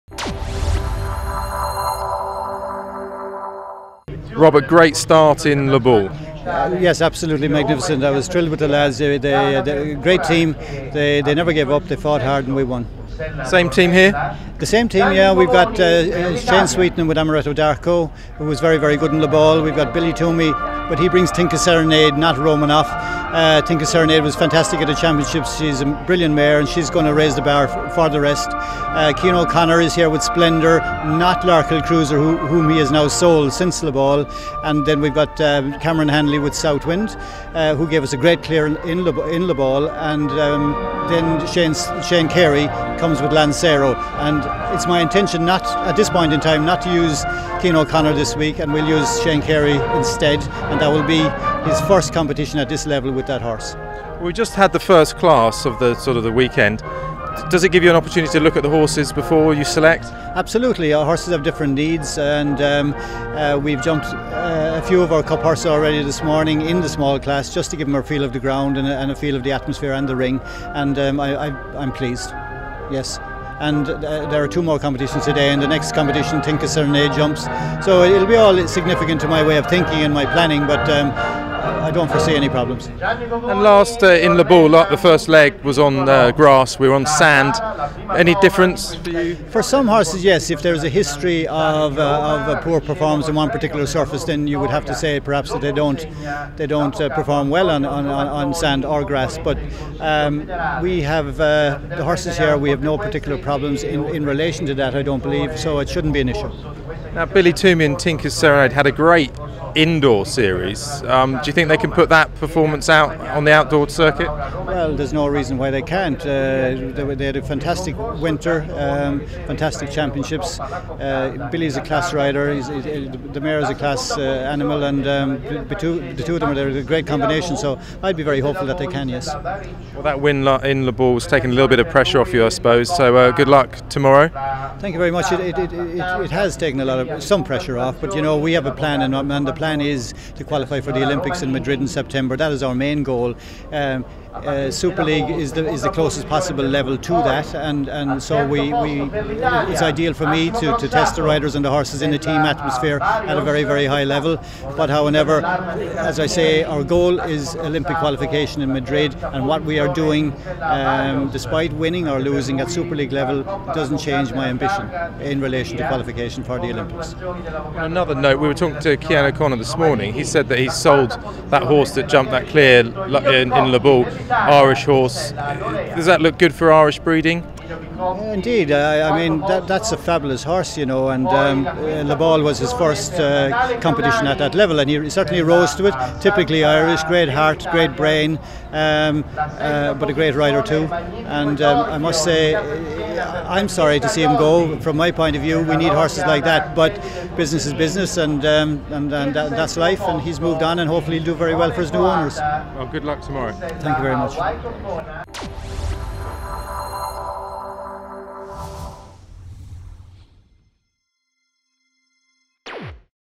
FEI Nations Cup 2011 - Rome, Robert Splaine
After the best start to season Chef d'Equipe of Ireland Robert Splaine talks us through his team selection